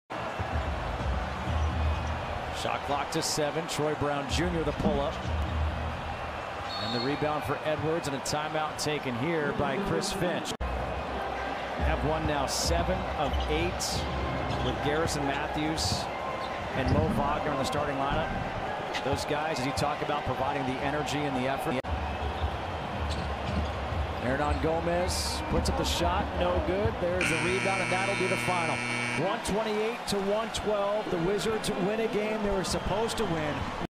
Troy Brown Jr. : All Possessions (2021-02-27)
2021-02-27 Washington Wizards vs Minnesota Timberwolves: Player Highlights Troy Brown Jr.
    0 PTS, 1 REB, 0 AST, 0 STL.
        0 of 0 from 3.
        0 of 2 FGs in 4:24 minutes.